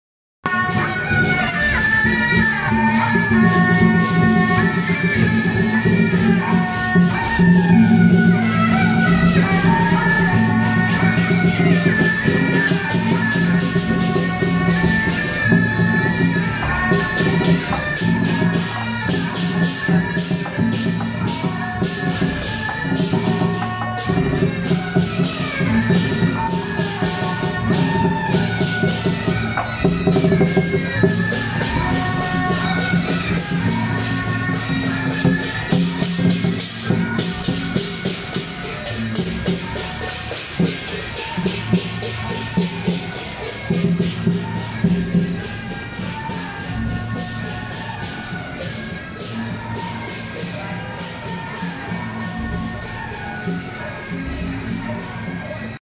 【鶯歌德川-醒獅團】~109中和皇明宮中壇元帥南巡謁祖回駕暨35週年遶境
鶯歌德川-醒獅團